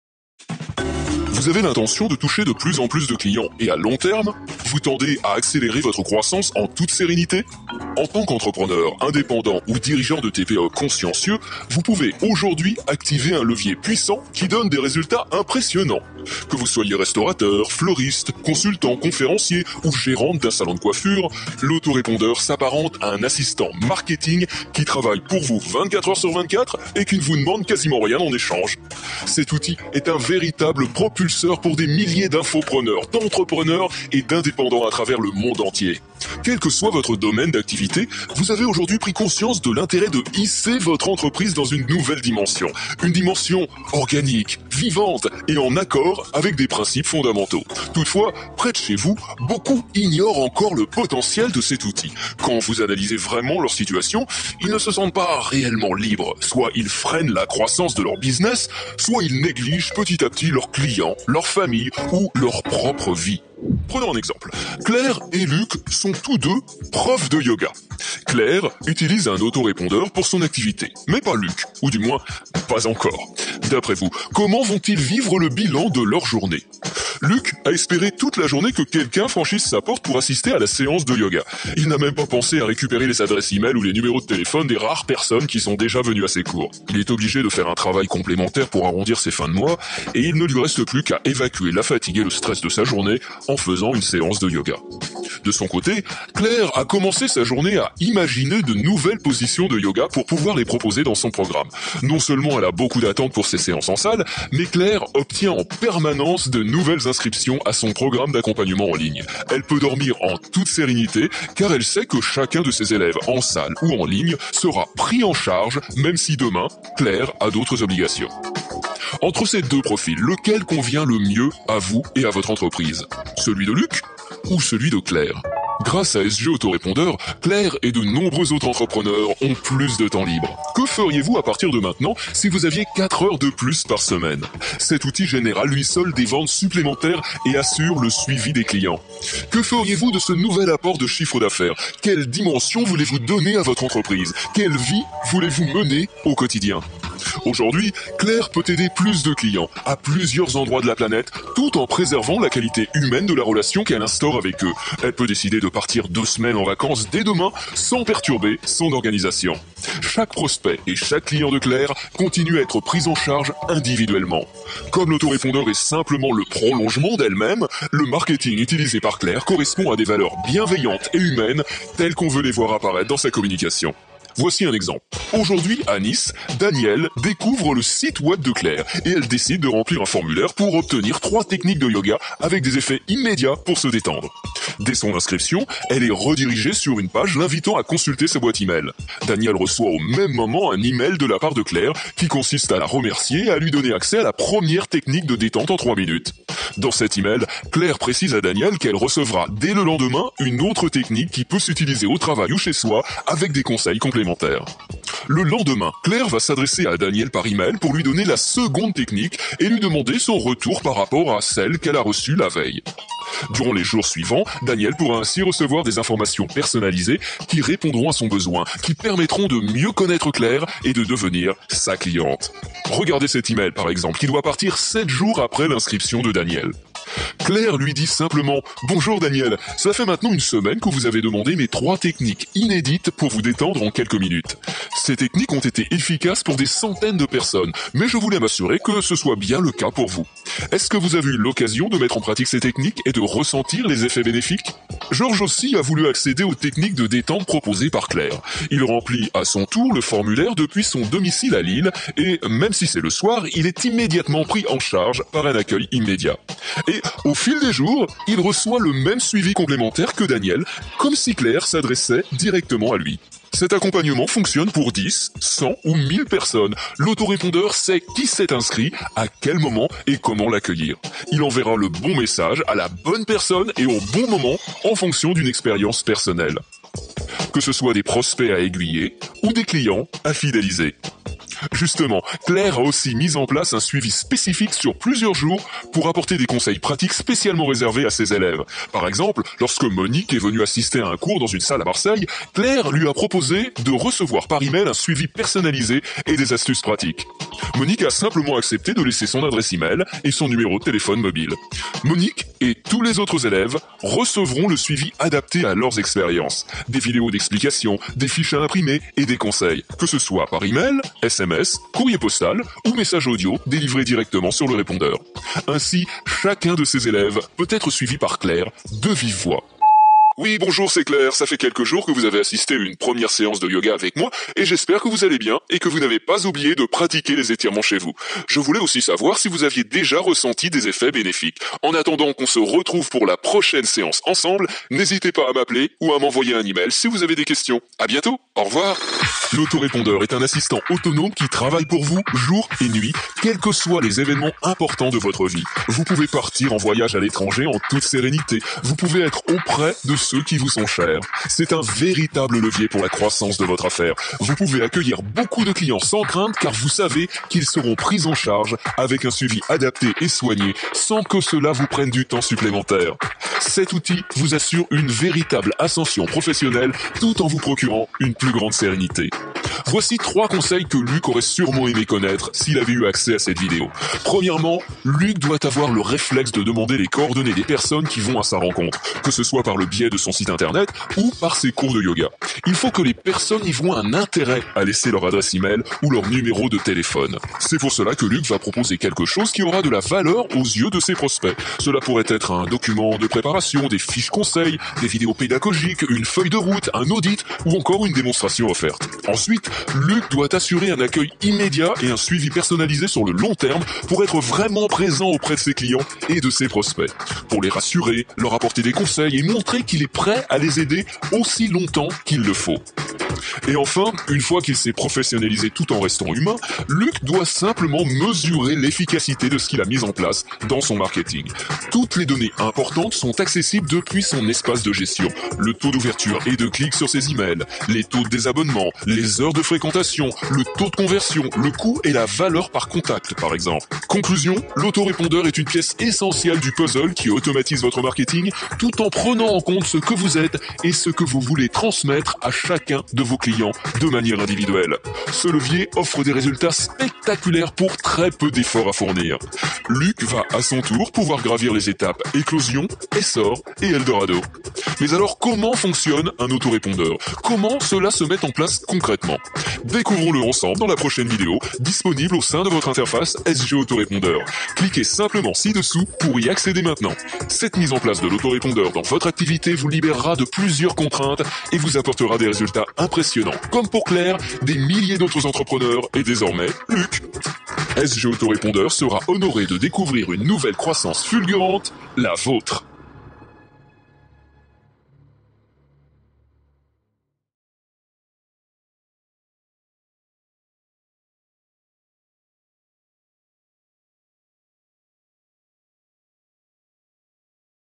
Autorépondeur email gratuit : SGAR (voir description)
On va le faire en plus long :

- Créé ta liste email et tout le tralala, séquence email, pages de captures, etc
- Dans la signature de tous les emails de ta séquence, tu mets une petite phrase et ton lien affilié vers l'autorépondeur, juste dire j'envois avec SGAR parce que c'est le meilleur, un truc comme ça
- Tu fais ton job normal, emmener des gens à s'inscrire sur ta liste email pour recevoir tes emails
- Au bout d'un autre moment tu auras plus de filleuls et tu pourras quitter ton emploi, ta femme et enfants, ton pays et vivre en thailande avec des happy endings tous les jours jusqu'à ta mort
- Problème, ça te fera chier de mourir, mais on doit tous en passer par là